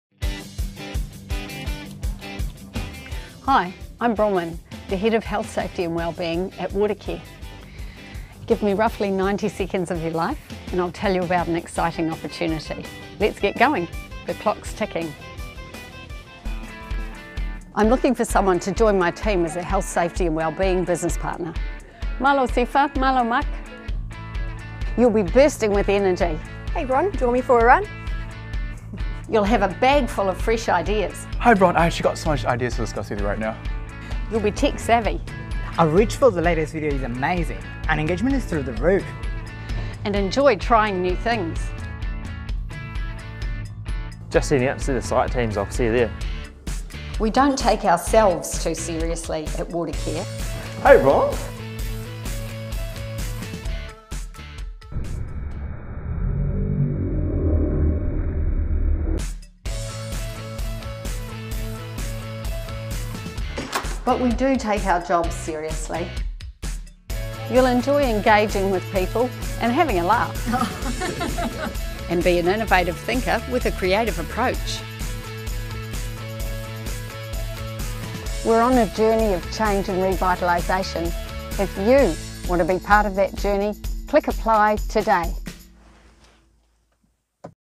New year, new you! Are you the next Watercare team member?
We all know the saying 'new year, new you'! Well, maybe our exciting role of health, safety and wellbeing business partner is the new you you've been searching for! Press play to find out what you'll need to bring to the table to succeed at Watercare.